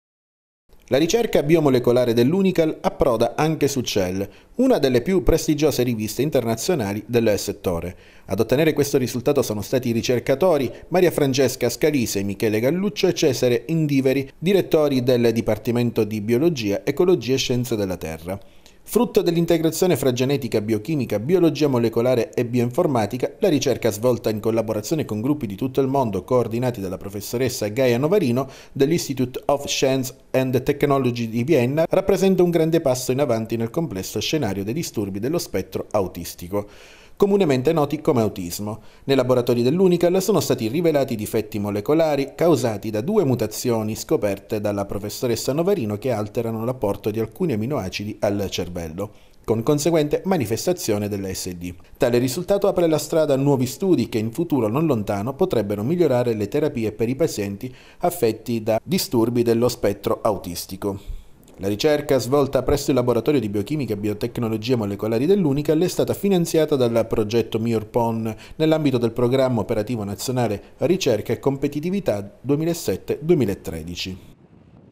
RICERCA BIOMOLECOLARE SU RIVISTA CELL
La ricerca Biomolecolare dell’UNICAL approda anche su "Cell", una delle più prestigiose riviste internazionali del settore.